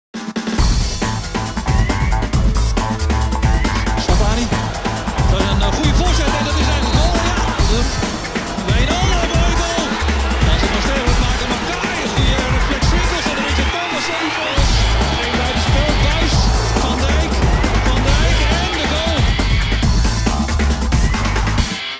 Feyenoord - Ado Den Haag 21-02-10 in 15 seconde
alle doelpunt achter elkaar